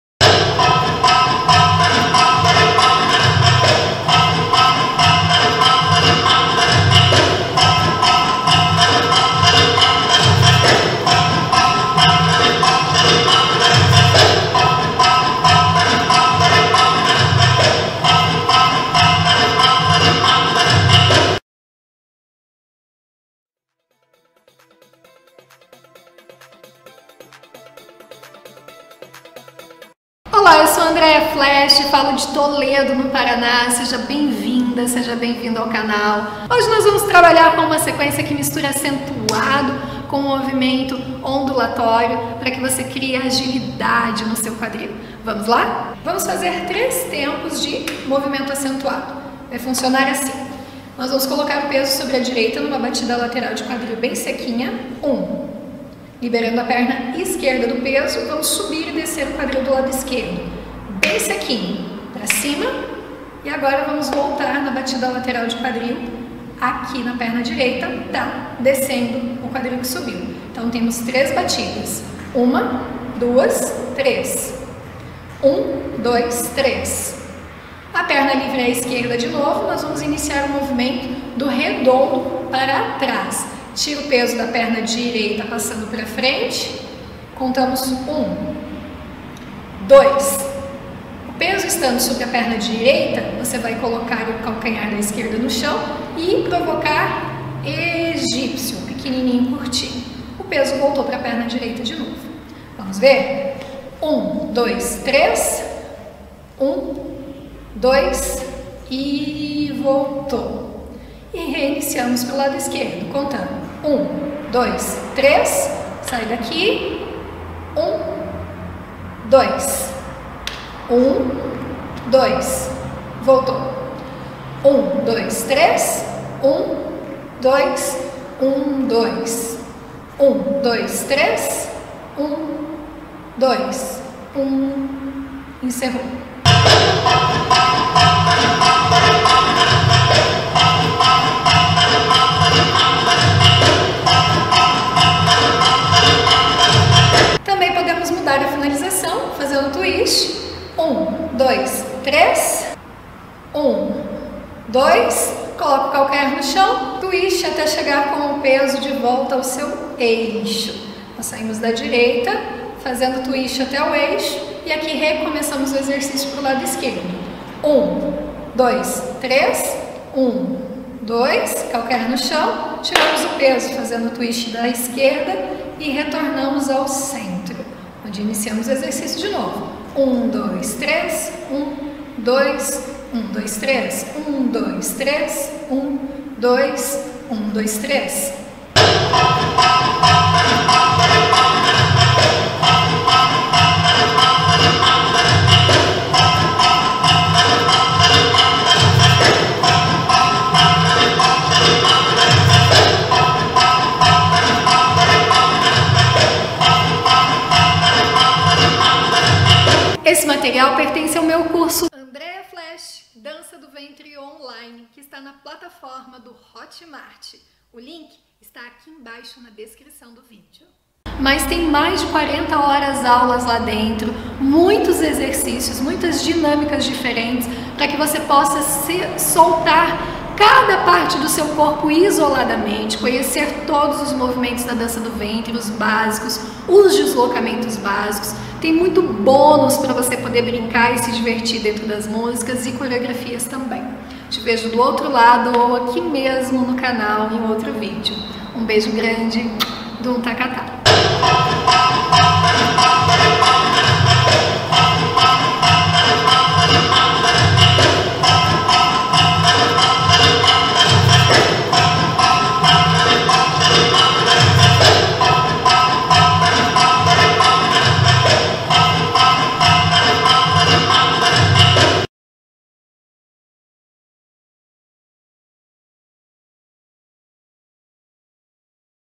Aulas de Dança do Ventre - Soltura de quadril II
Dois exercícios para soltar e agilizar o quadril.

Dum ta ka ta para voce.
Bons estudos para você!